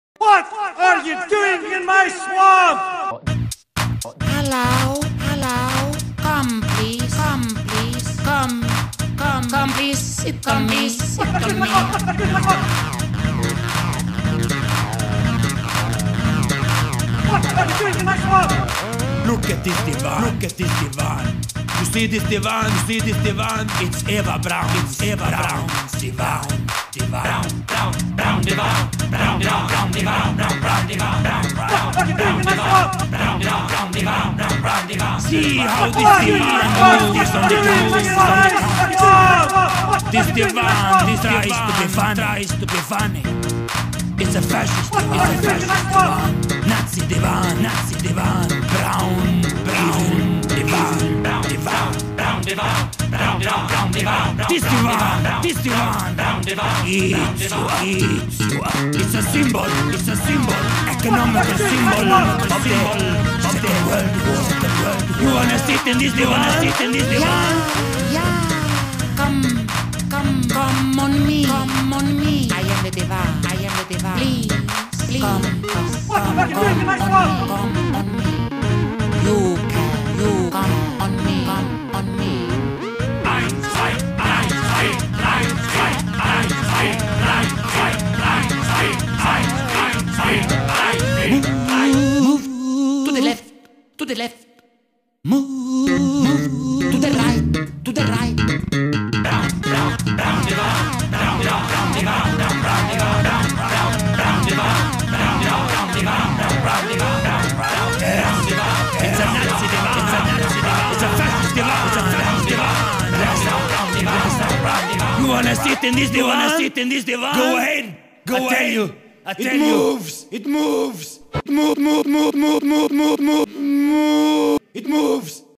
Braun Divan VS Shrek Swamp - ROUND 2
The Divan Is Fighting Some Shrek Today. Yea Its A Nazi Divan A Symbol Of The Second World War.